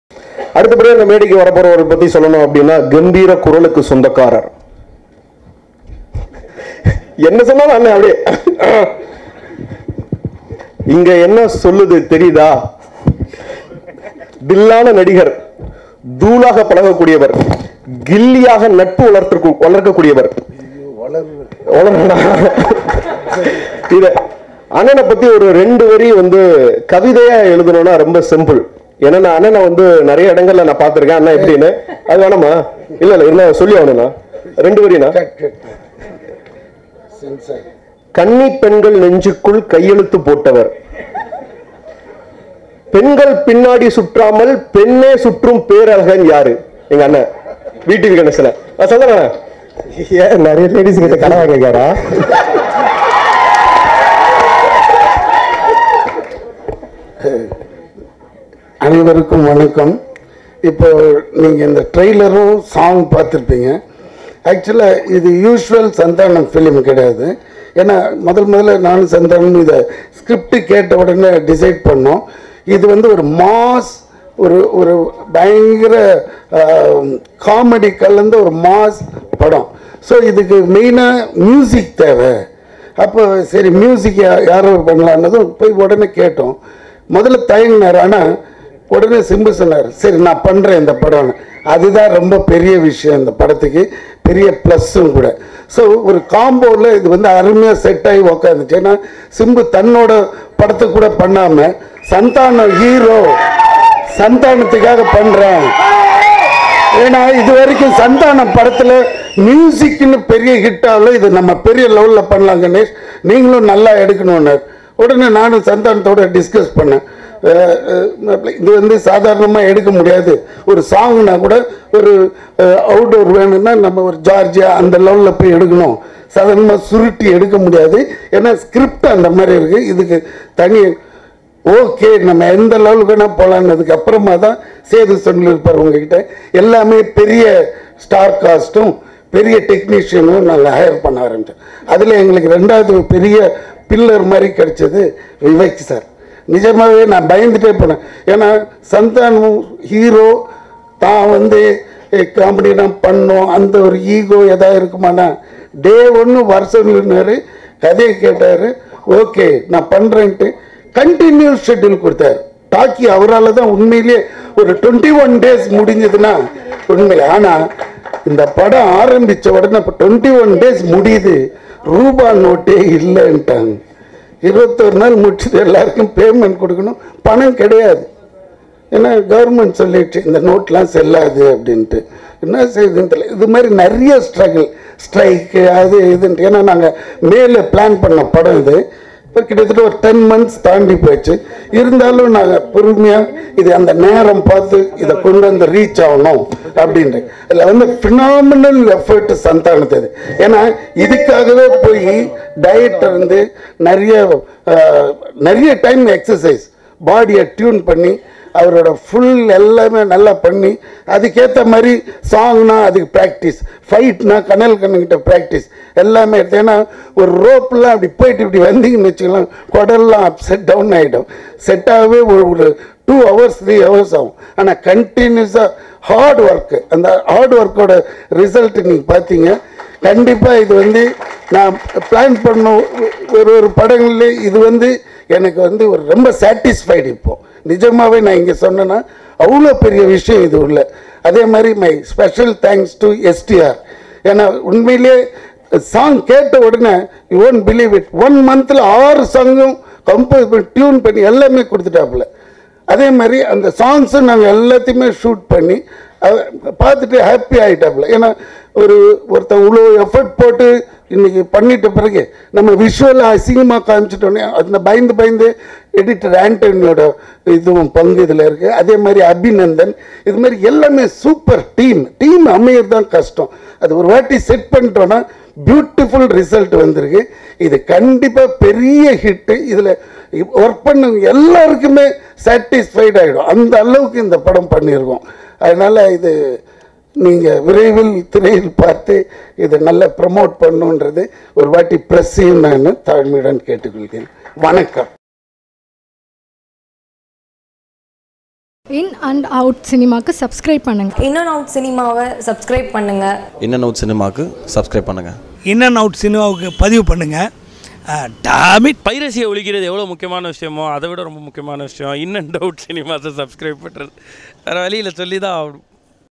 "STR Musical And Vivek Are The 2 Pillars Of This Movie" VTV Ganesh | Sakka Podu Podu Raja
STR Musical | Sakka Podu Podu Raja Trailer Launch

Movie - Sakka Podu Podu Raja
Cast - Santhanam, Vaibhavi Shandilya, Vivek
Music Director - STR
Director - G.L. Sethuraman
Edited by Anthony
Cinematography - Abinandhan Ramanujam
Producer - VTV Ganesh
Banner - VTV Productions